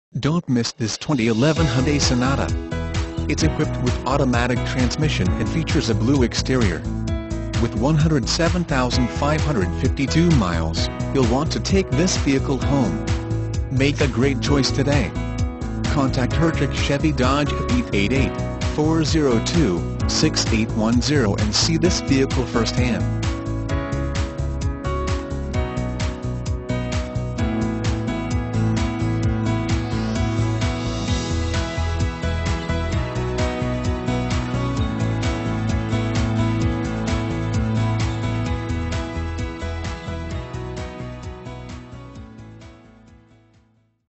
2011 Hyundai Sonata Denton MD 46322A
2011 Hyundai Sonata SE - $14495

Hertrich Chevy Dodge
1123 Industrial Park Way
Denton MD 21629

Don't miss this 2011 Hyundai Sonata. It's equipped with Automatic transmission and features a BLUE exterior. With 107552 miles, You'll want to take this vehicle home. Make a great choice today.